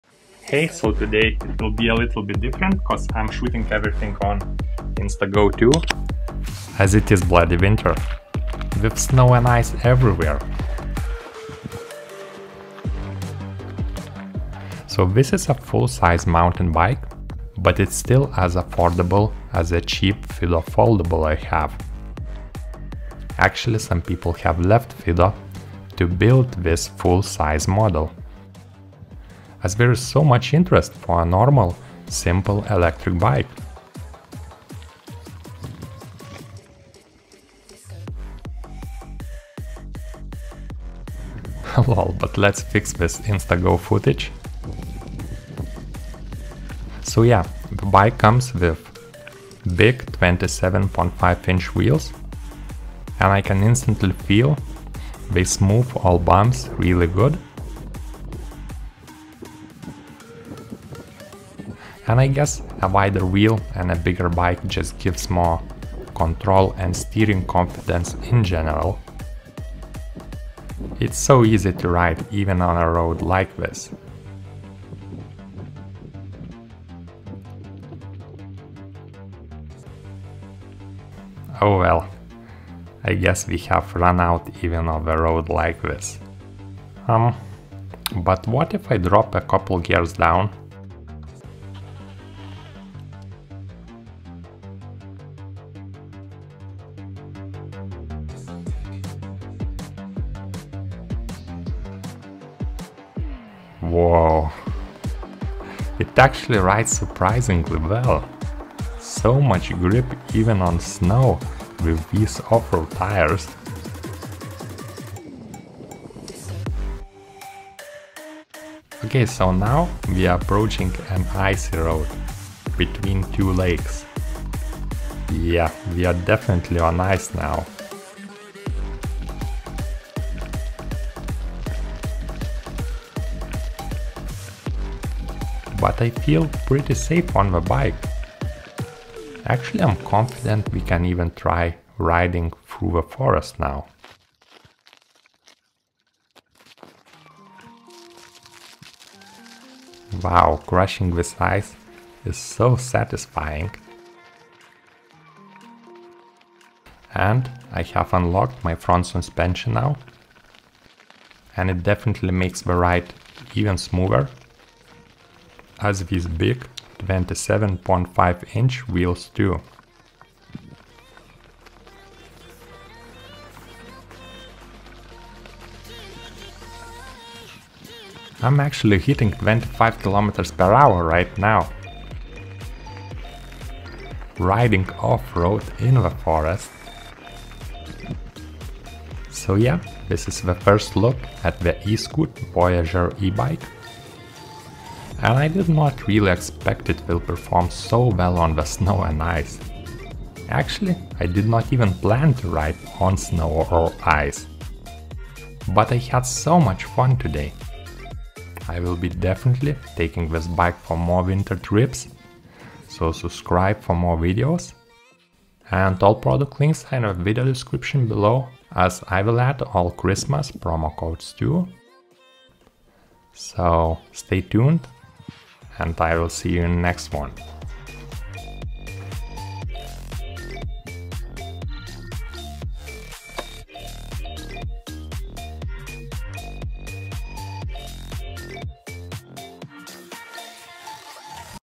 Riding in Winter ❄️ is EASY with 🔥Eskute Voyager Mountain Electric Bike!
Testing Eskute Voyager Electric Mountain Bike in Winter! I did not plan to ride the bike on snow and ice... but after trying and being suprised how well it handles on snow! I'm definitely will be going for more winter rides from now!

100 £ OFF or get an extra battery with the eskute bike purchase.

00:00 Eskute Voyager Intro
00:29 Riding Eskute Voyager
01:25 Riding electric bike on snow in winter
02:00 Riding ebike on ice in winter
02:25 Riding Eskute Voyager in a winter forest
03:10 Riding Eskute Voyager Conclusion

Voyager Mountain Electric Bike
"If you're looking for a E-Bike that stands out from the crowd, then the Voyager is it. From a distance you wouldn't know this is an E-MTB. Classic MTB lines, inline battery and rear hub motor give this electric mountain bike a great style."